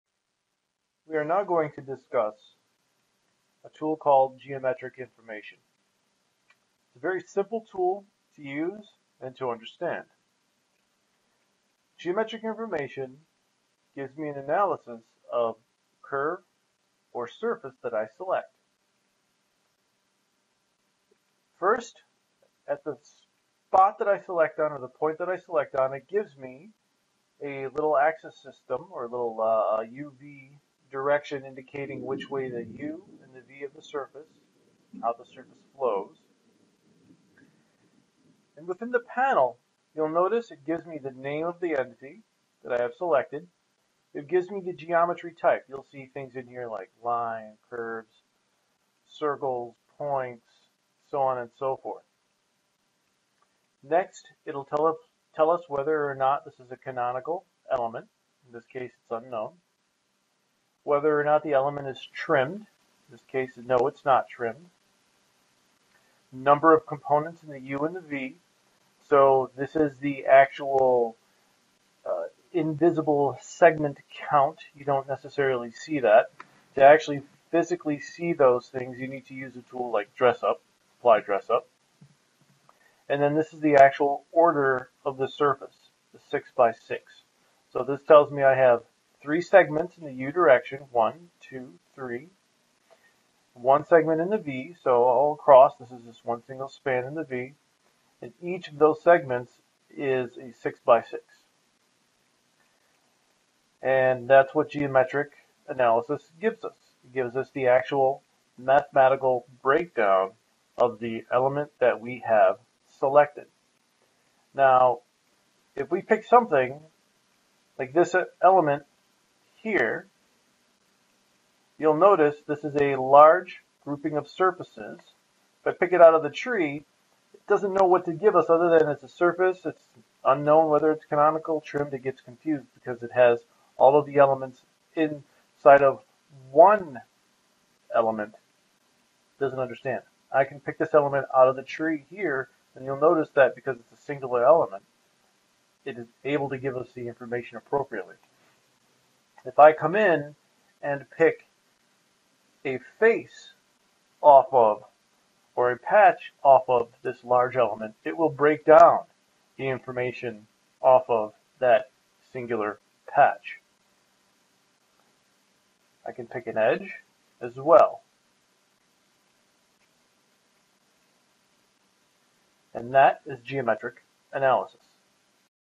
Catia V5 | Catia V6 Tutorial - Geometric Information
Geometric Information is a quick way to find out what the mathematical properties of curves and surfaces.